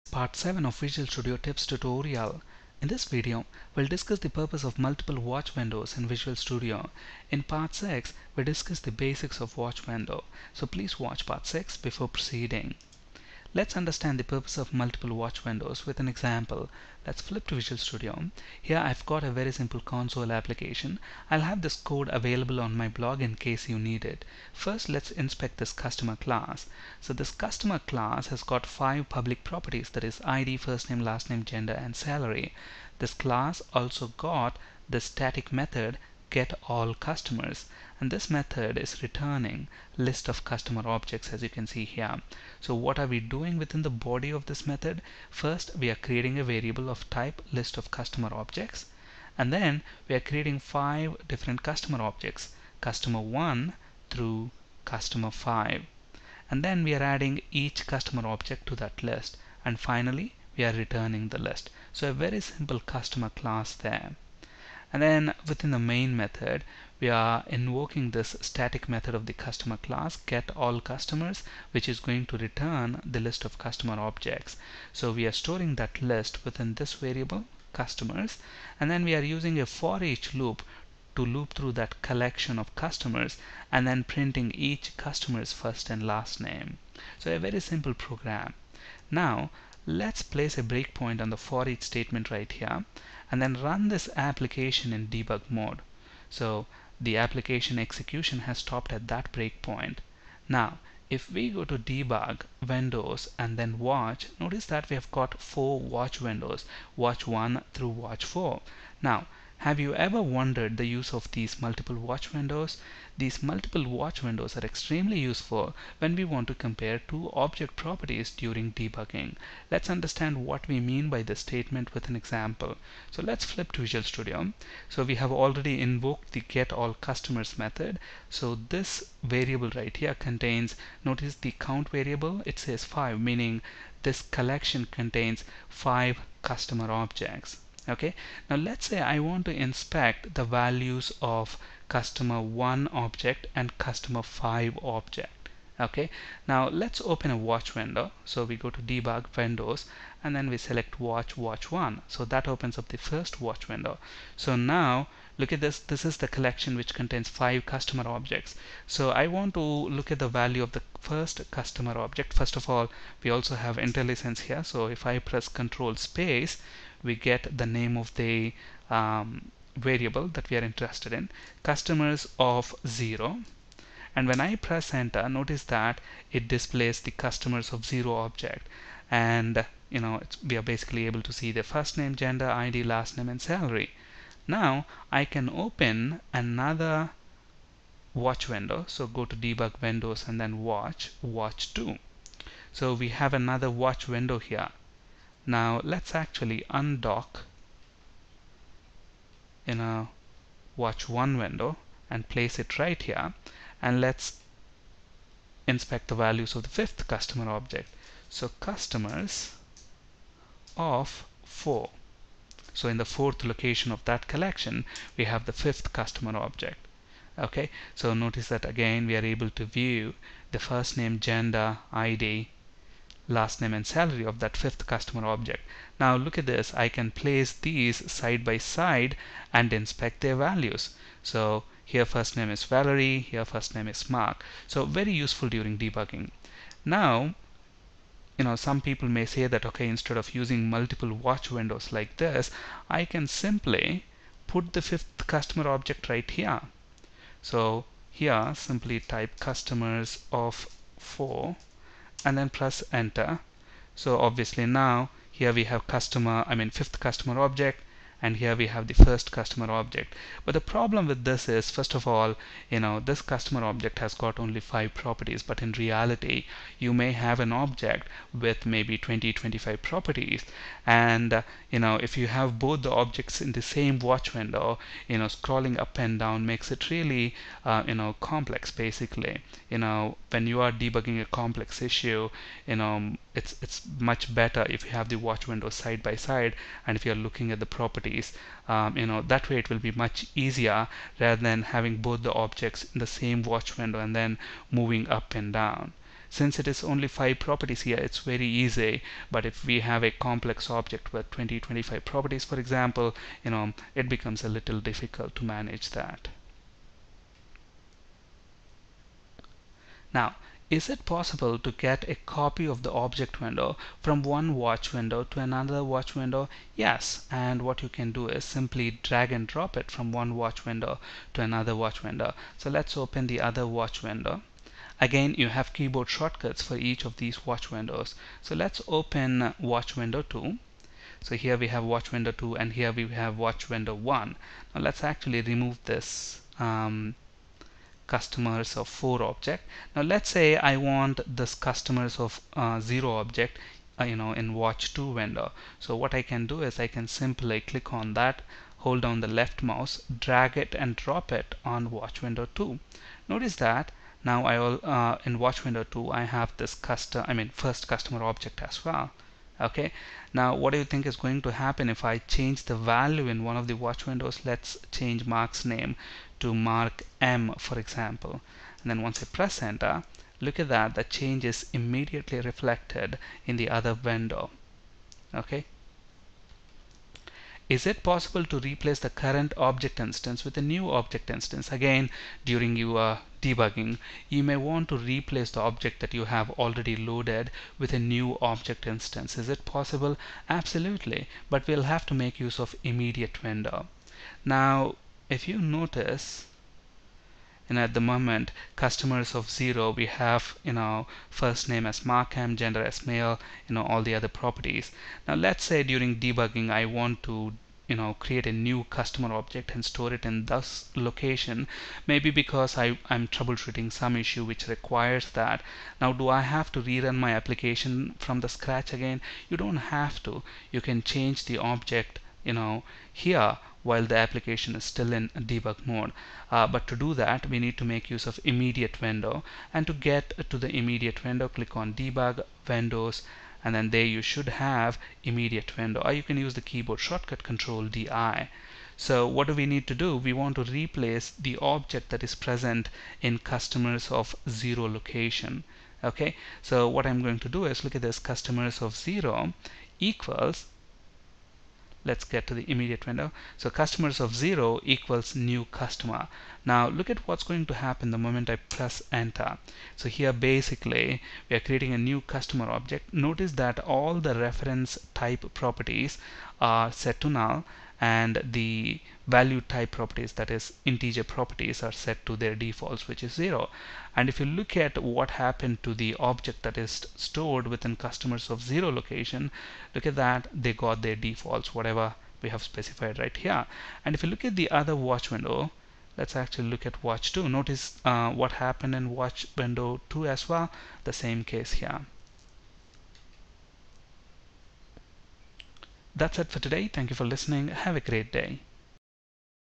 Part 7 Advanced debugging using watch window
When the application is in debug mode and if we navigate to Debug - Windows - Watch, you will see that there are 4 watch windows.

Have you ever wondered the use of multiple watch windows?
Multiple watch windows are useful when you want to compare 2 object properties during debugging.

Place a breakpoint on the foreach loop in the Main() method and run the application in debug mode. When the application execution stops at the break point, open 2 watch windows by going to Debug - Windows - Watch menu.

In Watch 1 window, type customer[0] in Name column and press Enter key. Along the same lines in Watch 2 window, type customer[1] in Name column and press Enter key. Notice that now you have the opportunity 2 inspect 2 customer object properties.

Is it possible to get a copy of the object from one watch window to another watch window.
Yes, simply drag and drop. If you change a property of the object in one watch window, the change will be automatically reflected in the other watch window.

Is it possible to replace the current object instance with a new object instance
Yes, simply create a new object instance in immediate window. For example the Watch 1 window below displays the first customer object in customers collection.

To replace the current Customer object in customers[0] location. Open Immediate window by going to Debug - Windows - Immediate. Type the following command in Immediate window and press Enter key.
customers[0] = new Customer()

Notice that the Watch 1 windows now displays, the default values of the new Customer object that we have created.